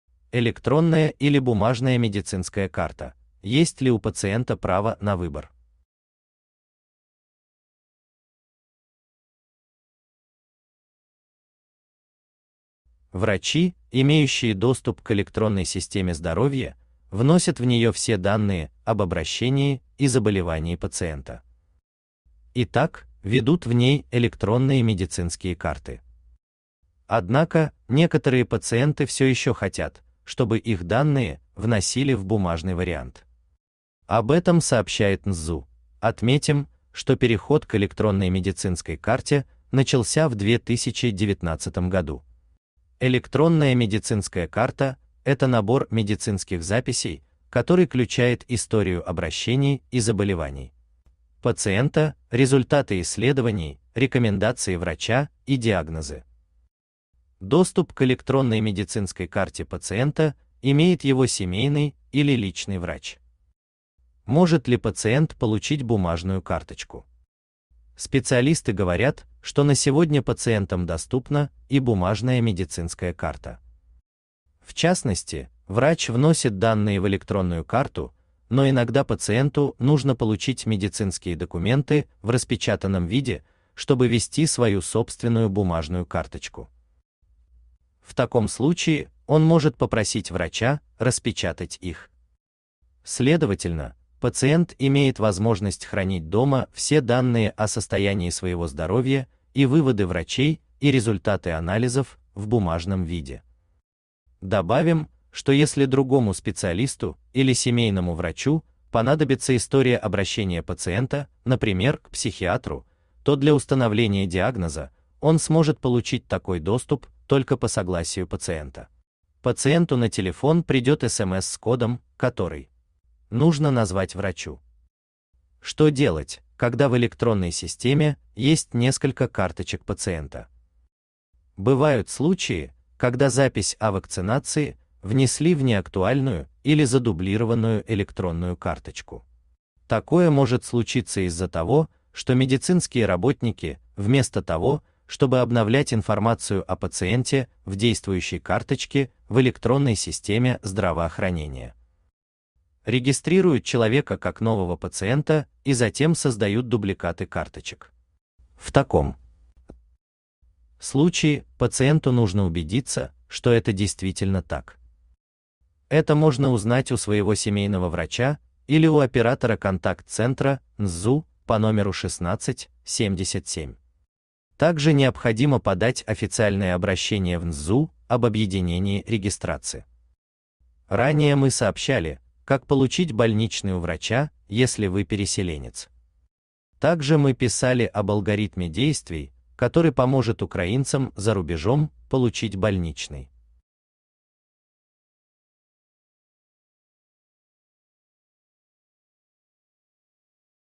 Электронная или бумажная медицинская карта: есть ли у пациента право на выбор.
больница врачи больничный Врачи, имеющие доступ к Электронной системе здоровья, вносят в нее все данные об обращении и заболевании пациента. Итак, ведут в ней электронные медицинские карты. Однако, некоторые пациенты все еще хотят, чтобы их данные вносили в бумажный вариант. Об этом сообщает НСЗУ. Отметим, что переход к электронной медицинской карте начался в 2019 году. Электронная медицинская карта - это набор медицинских записей, который включает историю обращений и заболеваний пациента, результаты исследований, рекоменда